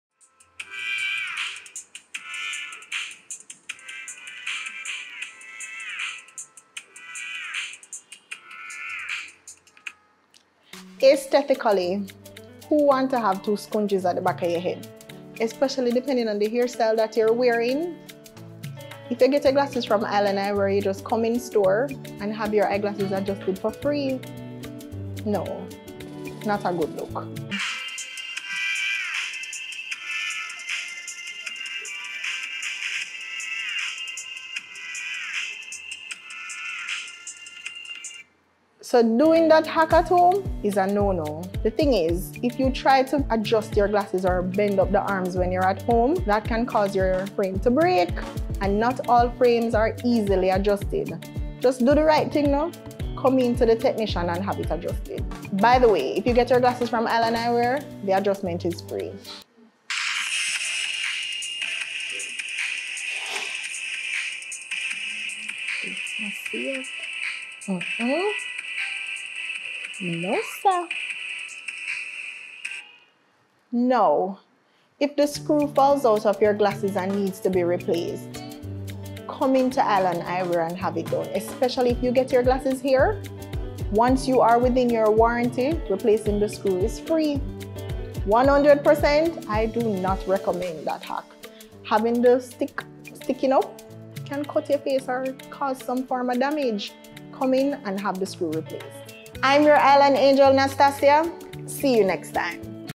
Buzzfeed's Glasses Hacks Every Four-Eyes Needs To Know REACTION VIDEO | Eyeland Eyewear
It's a no from us!

We do not recommend trying any of these 'tips' to fix your frames. Play it safe and visit us today for quality eyeglasses and excellent customer service!

Our Eyeland Angel Nastasia is always here to help!